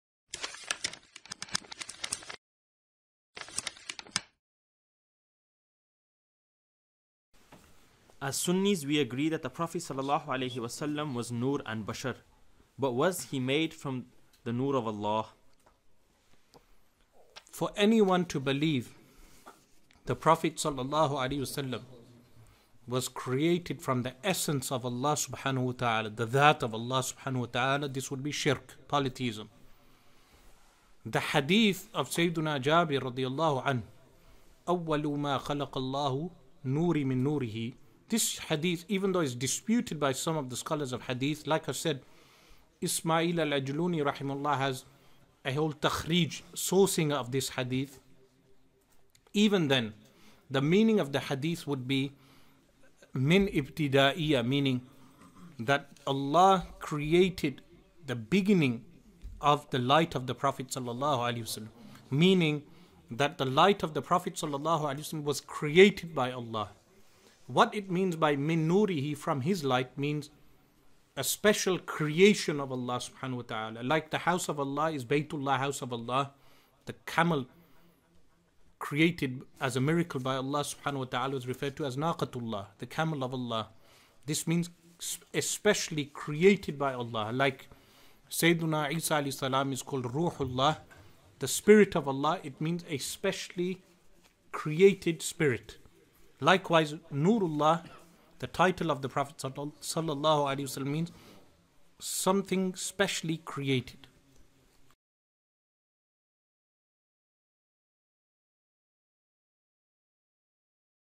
Is Prophet Muhammad ﷺ made from the Noor of Allah?
Green Dome Trust presents - Shaykh Asrar Rashid answering questions posed by students at the SeekersRetreat2017.